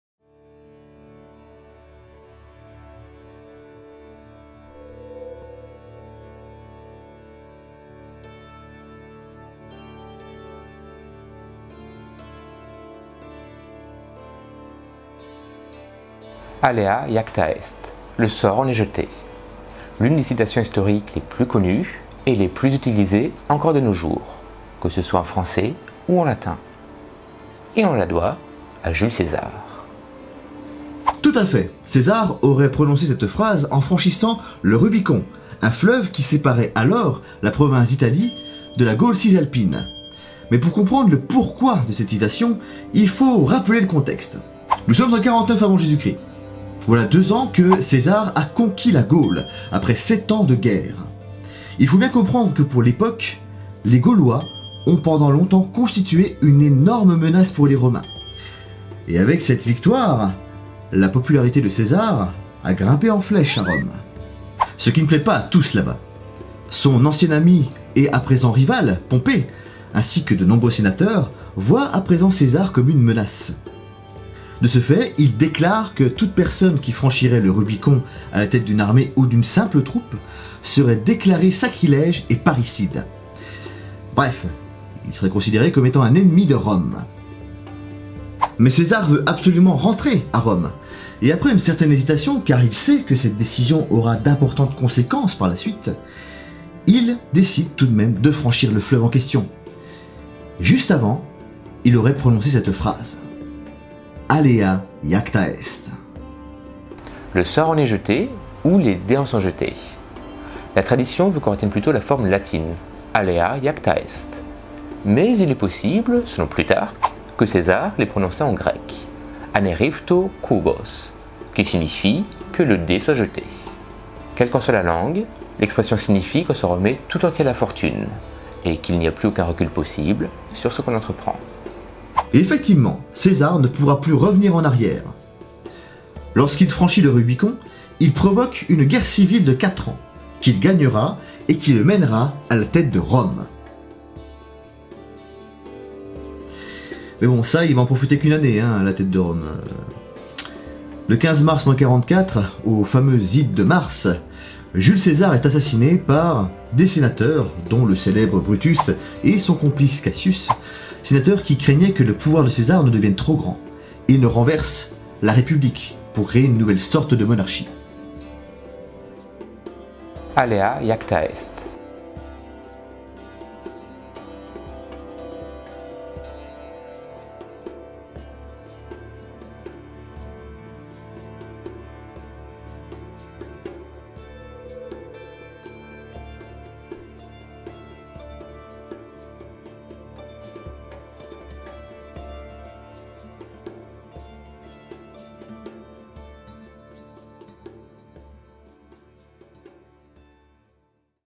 Alea Jacta Est - L'Histoire avec une grande Arche (feat. L'Arche)
D'où vient cette expression "Alea jacta Est" ? Et que signifie-t-elle ? Voici deux questions à laquelle deux personnes vous répondent de vive voix.

Bibliographie utilisée :
CAIUS JULIUS CAESAR, La Guerre des Gaules ;
CAIUS JULIUS CAESAR, La Guerre Civile ;
L. FEZZI, Alea Jacta Est, Belin, 2018 ;
M. Le Glay, J.-L. Voisin & Y. Le Bohec, Histoire romaine, PUF, 2011 ;
P. COSME, Auguste, Coll. Tempus, Perrin, 2005.